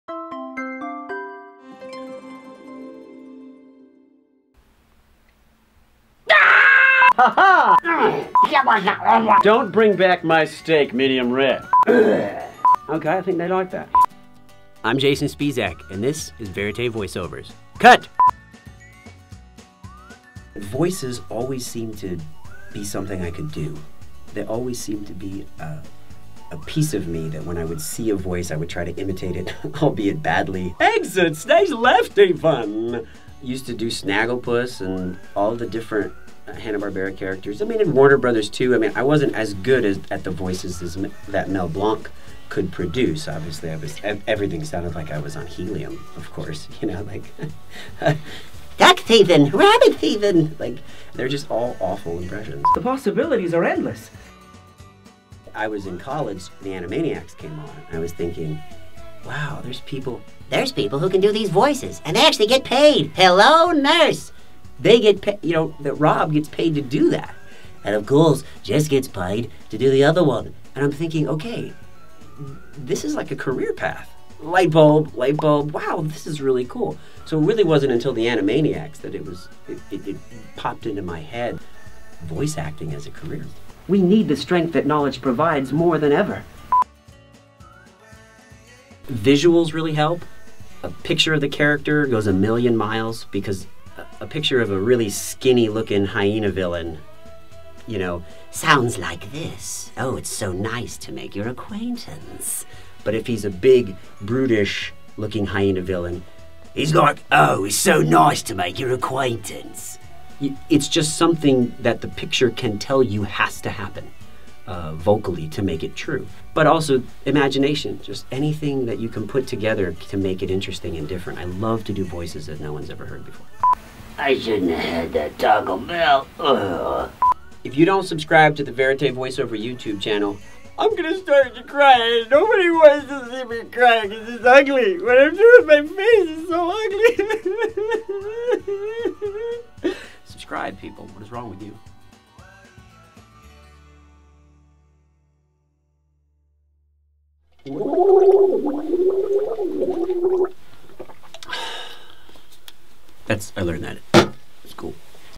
Jason Spisak | Talking Voices (Part 1)
Jason Spisak talks about discovering voiceover acting as a career, his love for impersonations, and finding the voices for his characters.